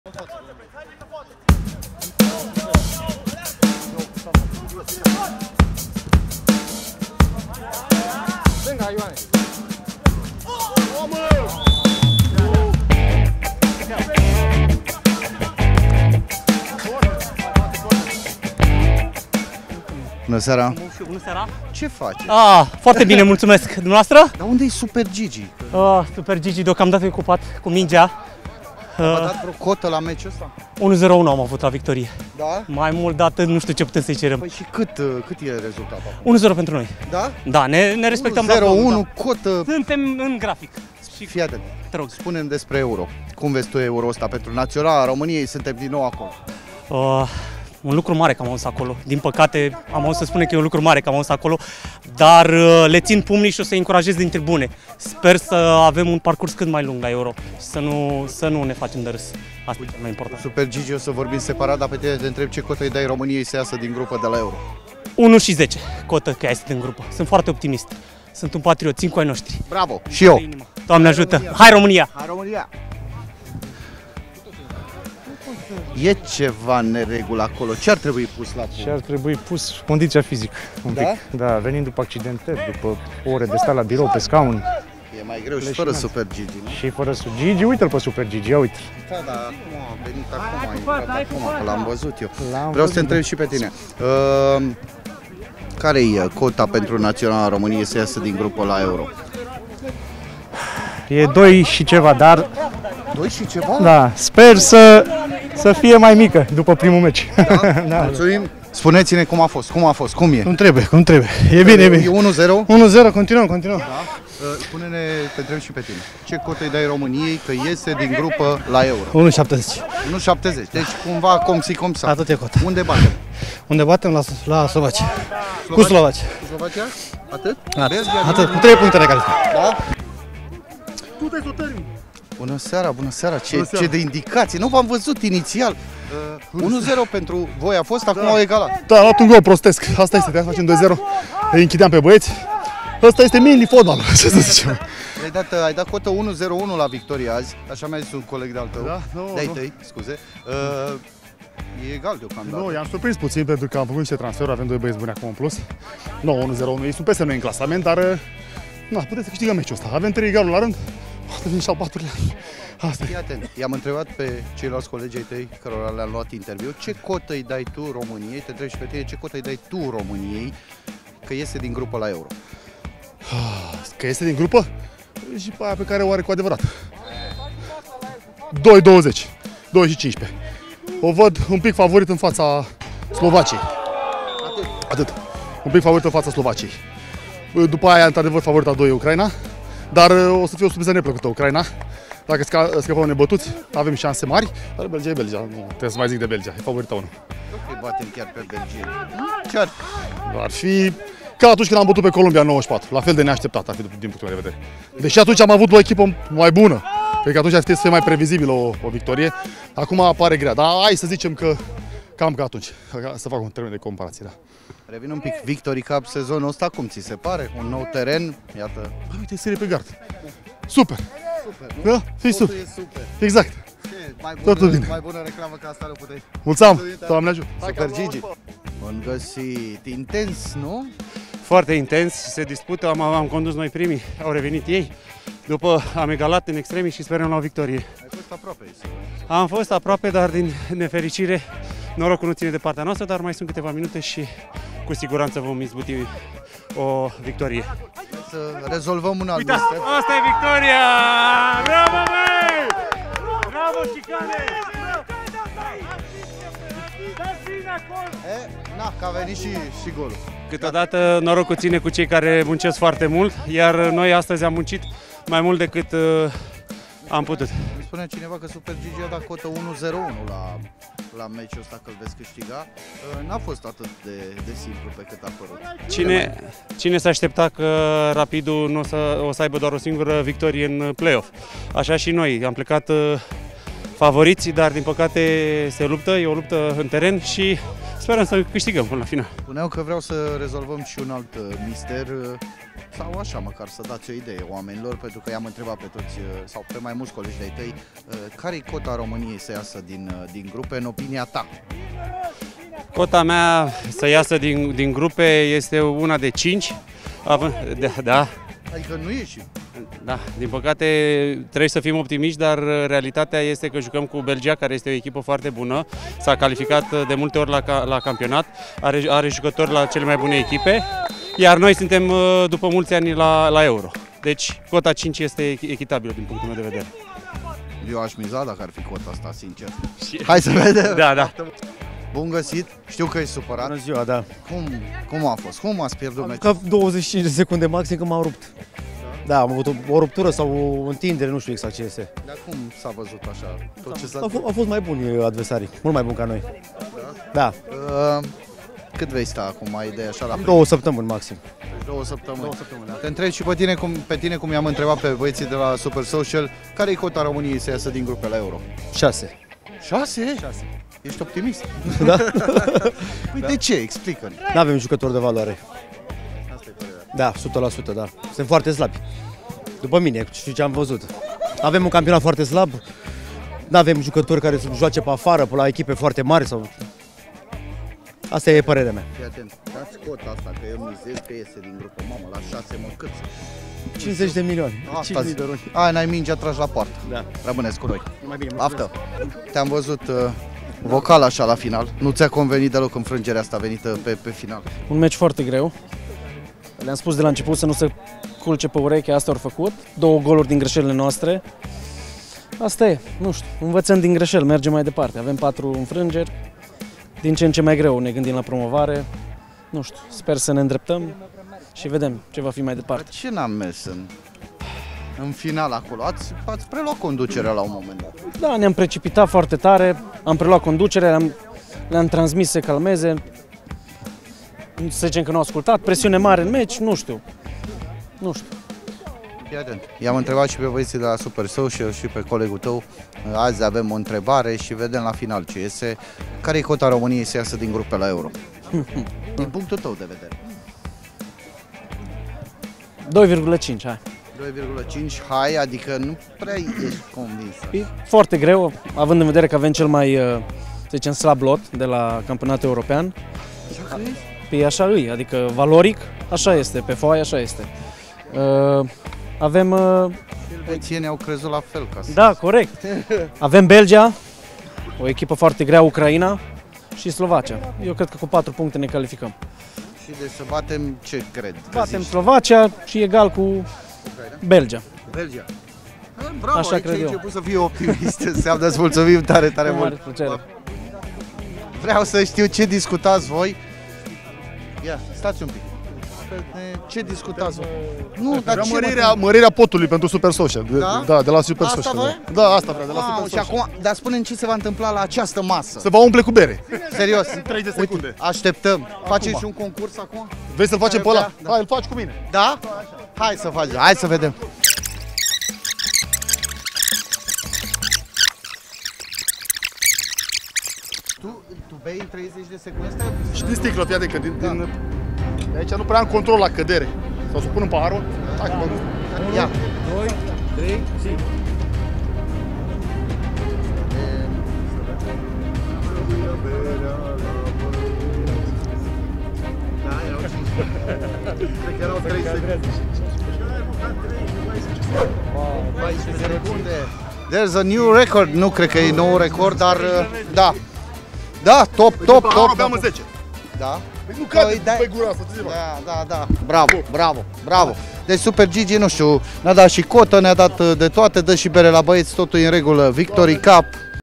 CE ȘANSE ARE ROMÂNIA LA EURO 2024 - VICTORY SHOW 3 | PARTEA 1
🎖️ 𝐅𝐈𝐈 𝐒𝐔𝐏𝐄𝐑 la Victory Cup, cea mai mare competiție de minifotbal din România, locul în care joacă peste 4.500 de persoane. Săptămânal se întâlnesc foști fotbaliști de renume, persoane publice sau pasionați ai fotbalului rege! 🔥

Episodul 3️⃣ din Victory Show ne găsește pe teren, iar reporterul nostru, Marian Codirlă ia pulsul fanilor înainte de EURO 2024 și aflăm părerile oamenilor cu privire la echipa națională a României! 🇷🇴

SuperGigi analizează grupa de la Campionatul European și este optimist cu privire la calificarea tricolorilor. După victoria echipe sale, SuperSocial, Gigi anunță că urmează sărbătoare la repriza a 3-a și răspunde afirmativ la provocarea de a termina o sticlă de bere într-un timp record! 🍻

Tu pe ce loc crezi că va termina România în grupa de la EURO? 👀